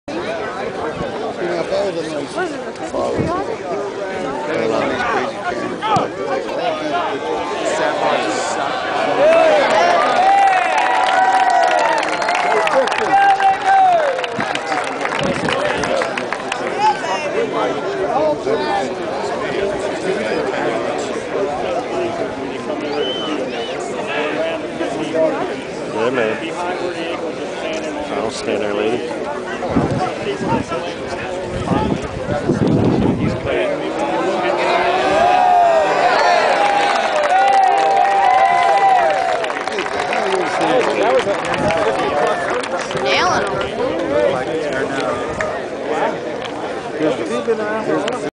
David Long Akers
Philadelphia Eagles Kicker @ Lehigh training camp on August 4th morning session nailing 60+ yarders..  The field position plus the 10 yards for the end zone plus at Lehigh there is another 10 yards to the goal posts. In keps fanyasy league it would be a 20 pt field goal. And remember mamas dont let your children grow up to be cowboys fans!